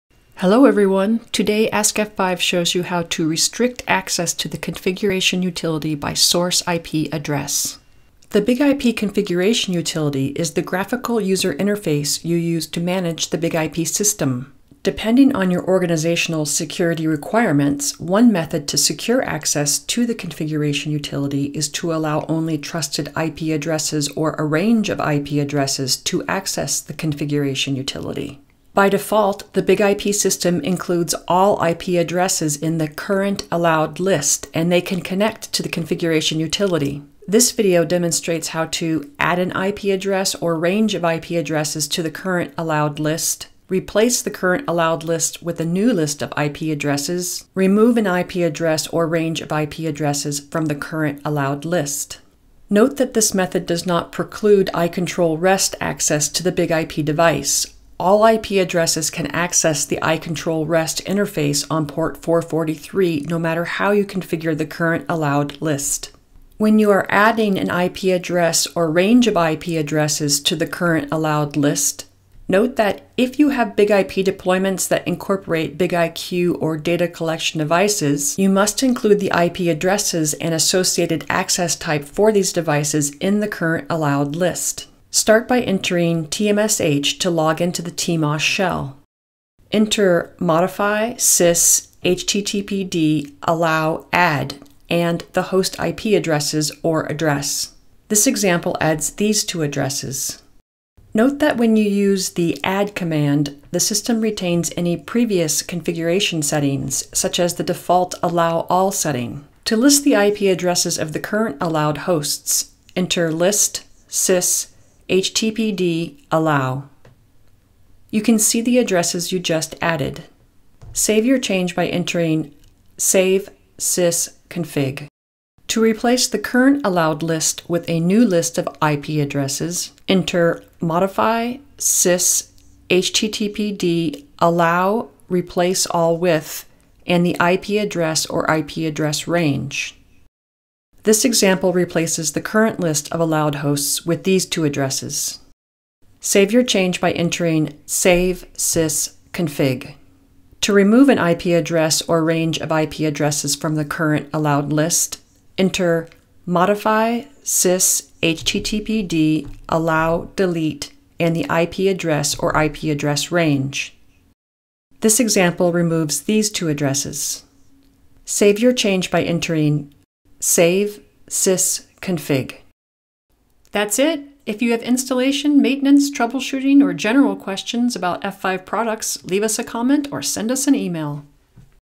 Restrict access to the BIG-IP Configuration utility by source IP address
The BIG-IP Configuration utility is the graphical user interface you use to manage the BIG-IP system. Depending on your organizational security requirements, one method to secure access to the Configuration utility is to allow only trusted IP addresses or a range of IP addresses to access the Configuration utility.

This demo uses BIG-IP 16.0.x

In this demo, AskF5 shows you how to:
00:00 Intro
01:06 Add an IP address or range of IP addresses to the current allowed list
02:05 Replace the current allowed list with a new list of IP addresses
02:30 Remove an IP address or range of IP addresses from the current allowed list

For more information and written procedures, refer to K13309: Restricting access to the Configuration utility by source IP address